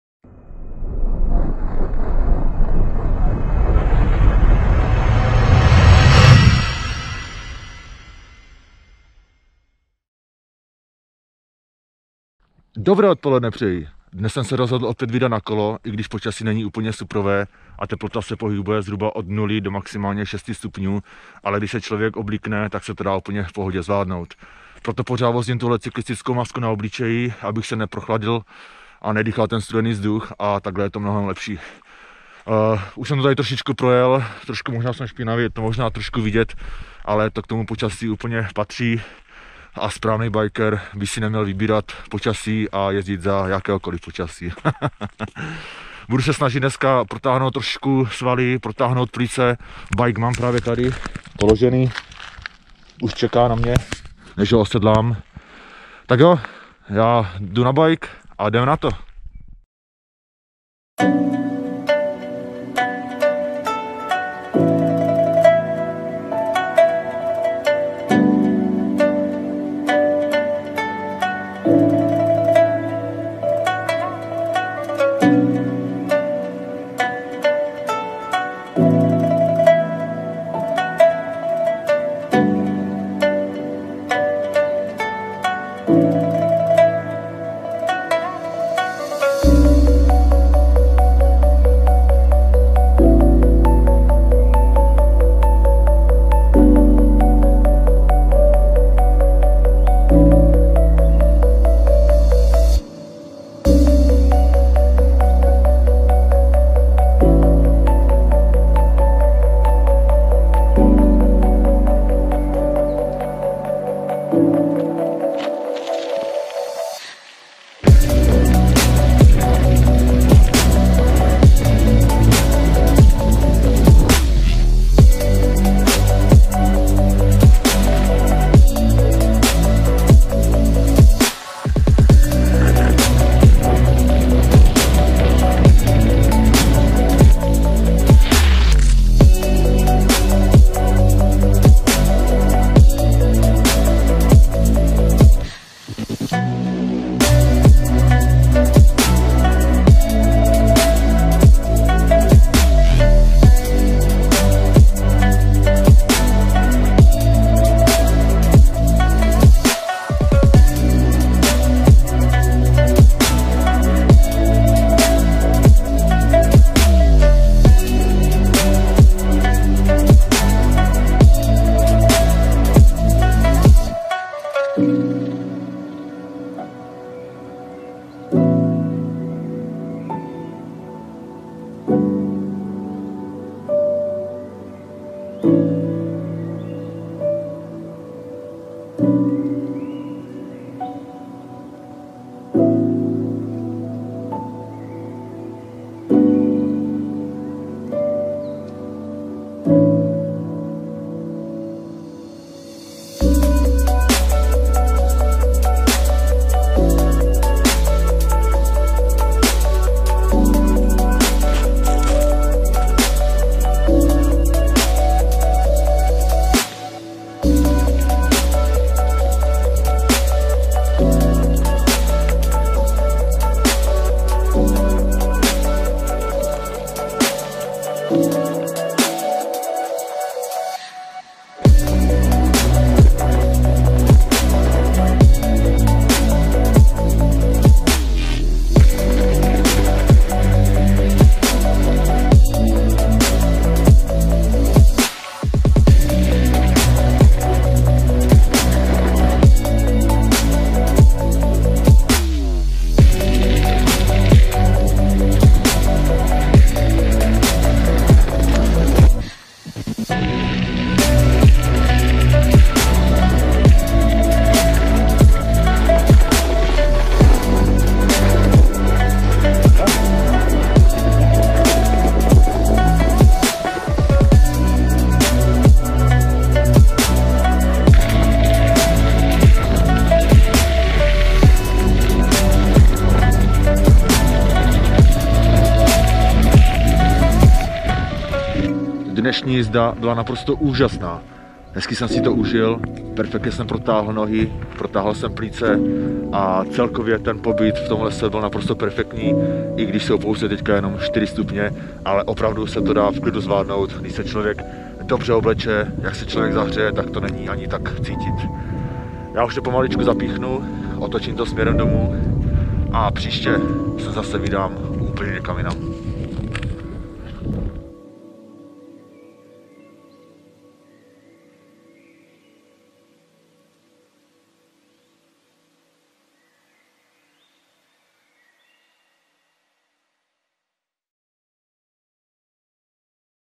První díl ze série NA KOLE : Březnový lesní terén
První díl ze série NA KOLE 🚵
který nese název : Březnový lesní terén.
Postupně se na kole budu vydávat do
zajímavých míst, ať už běžných lesních cest, různě veřejných lokací, národních přírodních památek,
vrcholů a samozřejmě i Bikeparků.
díl natočen dne 6.3.2021 🎬🎥